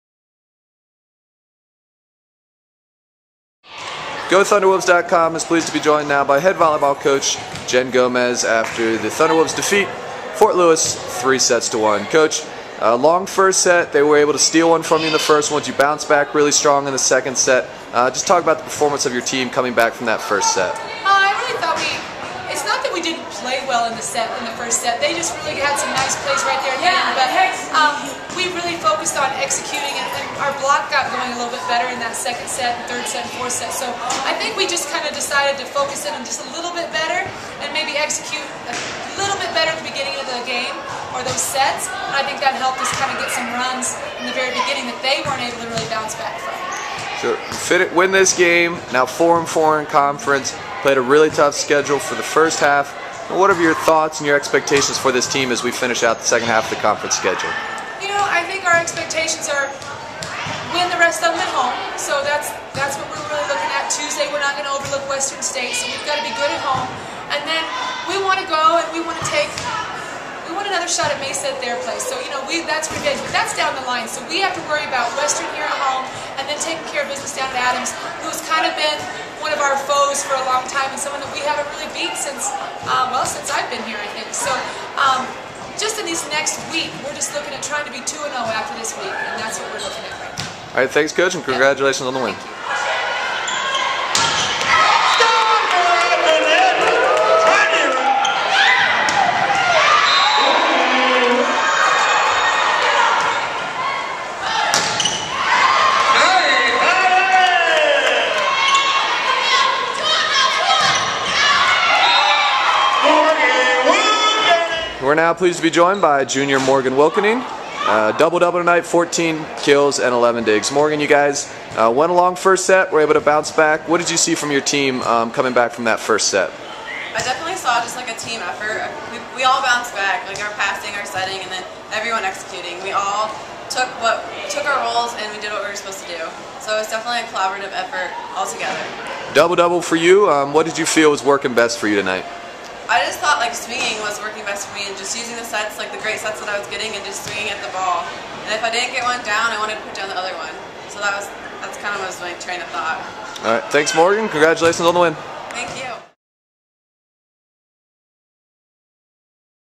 Volleyball: CSU-Pueblo vs. Fort Lewis
Postgame interviews and highlights from CSU-Pueblo's 3-1 win over Fort Lewis.